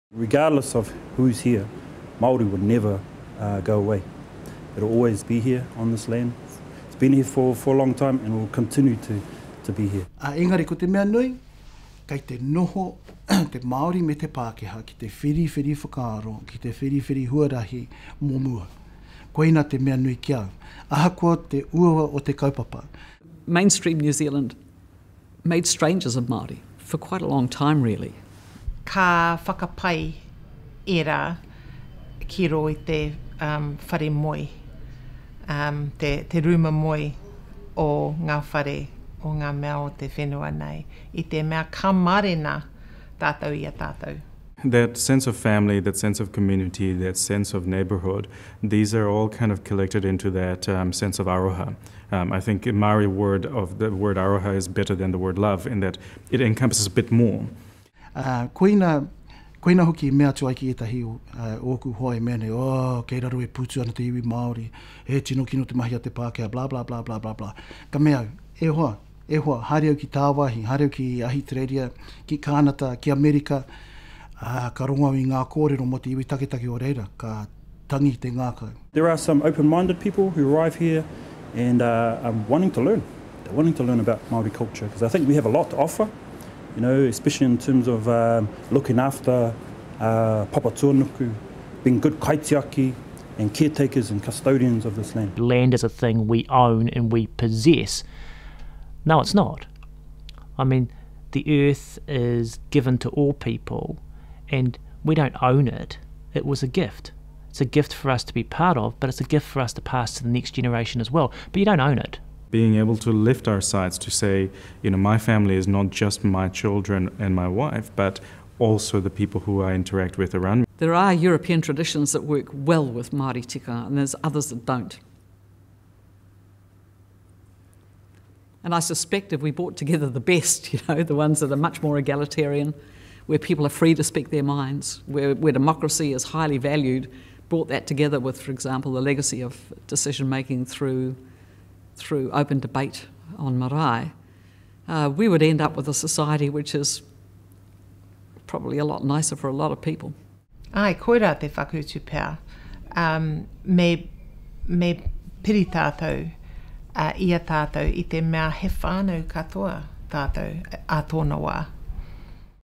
We can learn from each other
Hear a kōrero from the He Tohu exhibition about Te Tiriti with Ngāti Kawa Taituha, Ngahiwi Apanui, Prof Dame Anne Salmond, Judge Carrie Wainwright, Shamubeel Eaqub and Rt Rev Justin Duckworth.

"Regardless of who is here Māori would never go away. It will always be here on this land, it's been here for for a long time and it will continue to to be here."

He Tohu is a permanent exhibition of 3 iconic constitutional documents that shape Aotearoa New Zealand.

The documents are:

* 1835 He Whakaputanga o te Rangatiratanga o Nu Tireni – Declaration of Independence of the United Tribes of New Zealand

* 1840 Te Tiriti o Waitangi – Treaty of Waitangi

* 1893 Women’s Suffrage Petition – Te Petihana Whakamana Pōti Wahine

He Tohu is presented by Archives New Zealand Te Rua Mahara o te Kāwanatanga and the National Library of New Zealand Te Puna Mātauranga o Aotearoa, both of which are part of the Department of Internal Affairs.